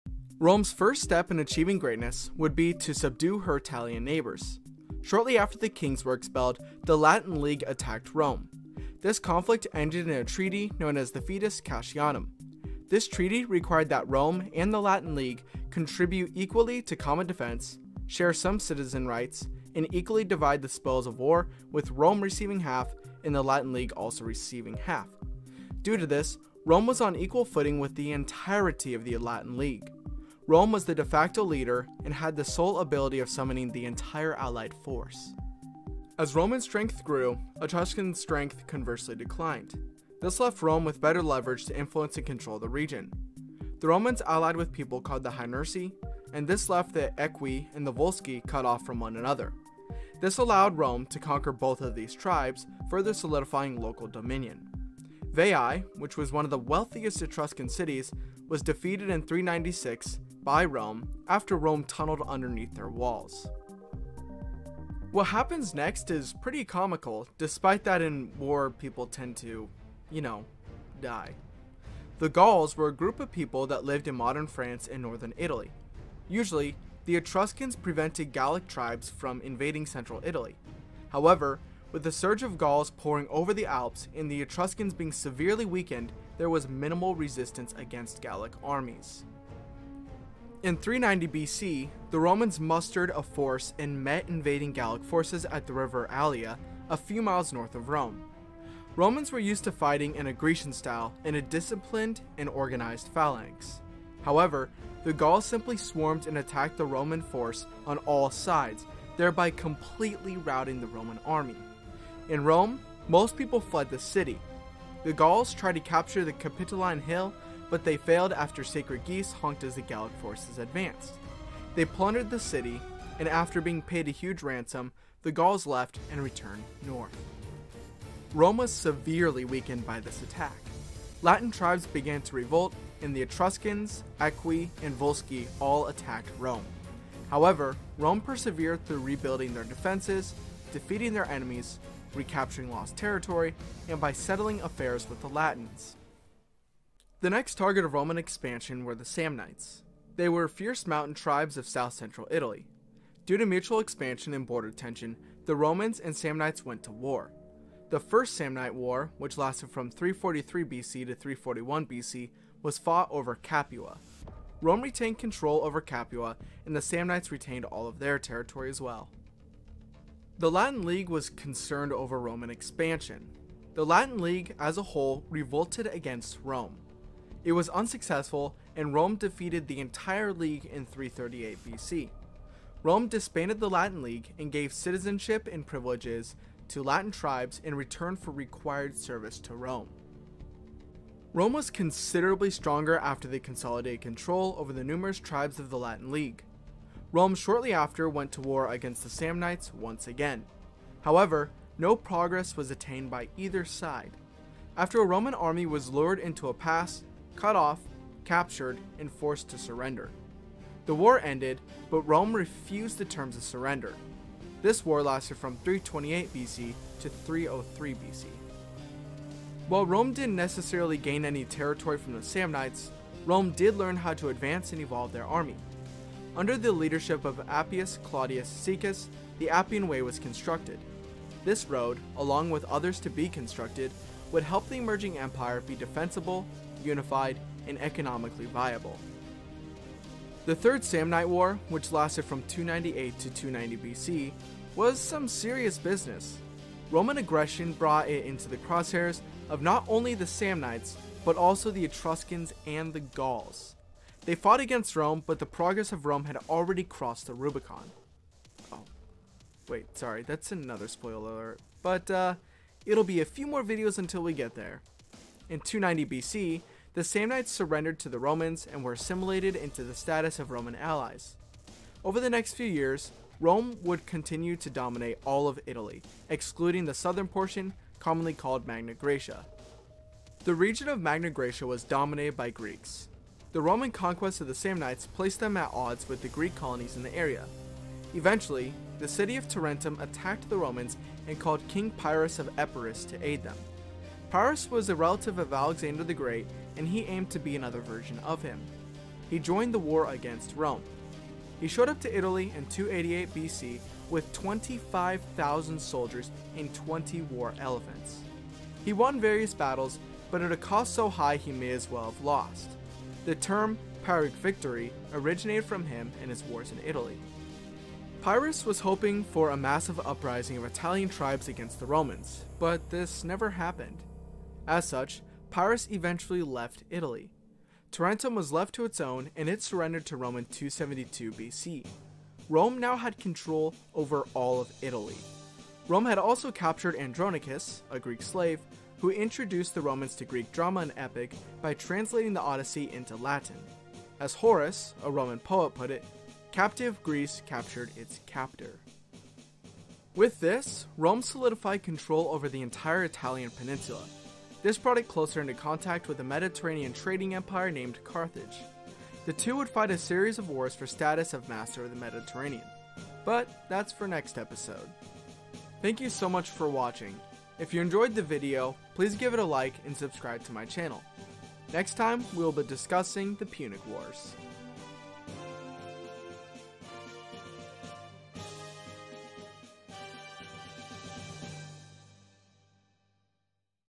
How Rome Conquered Italy
Learn in 6 minutes how Rome went from a fledgling republic into a regional powerhouse